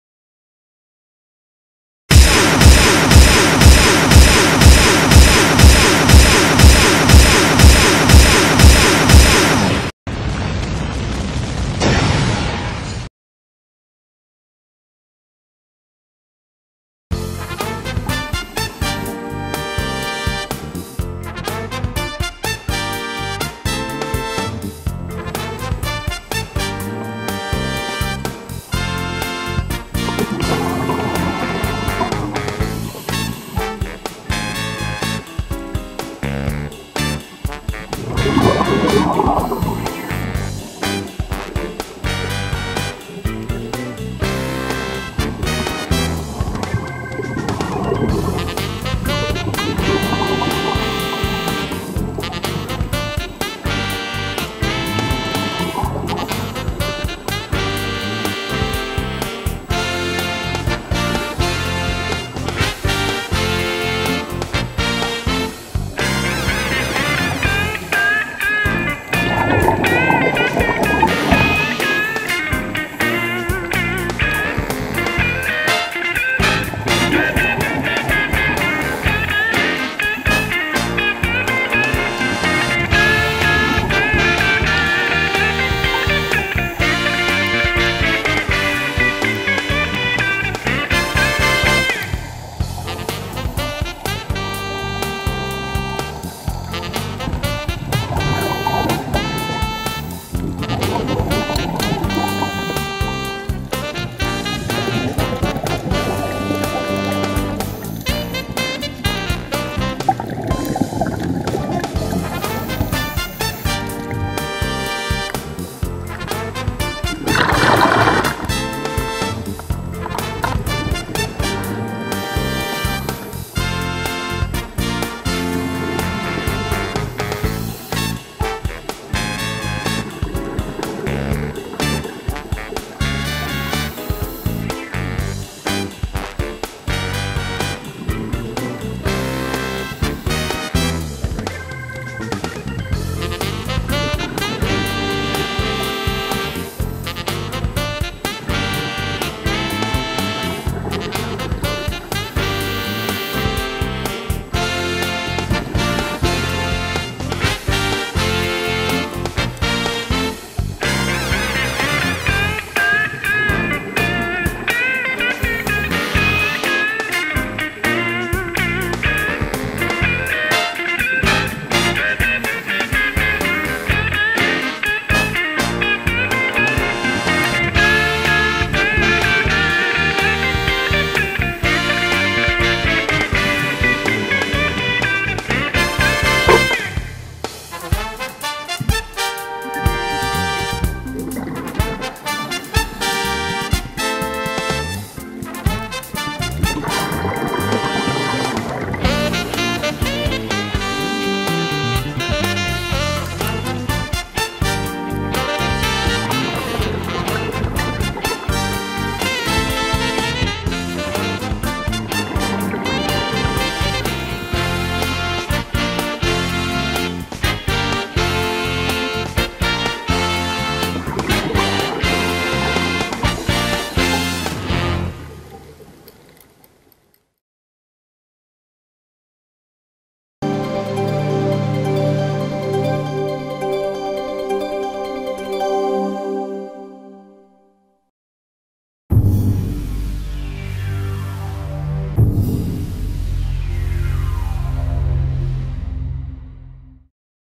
제주도3부-0909
시야 짱짱히터진 제주도. 연산호,전갱이,멸치떼.멸치떼를 습격하는 오징어,도화돔,문절,솔베감팽이,가시복어등 시야가 잘 나오니 수중 생물들도 다양하게 등장. 특히 범돔 Feeding은 오픈워터들에게는 쏠솔한 재미와 모험이었습니다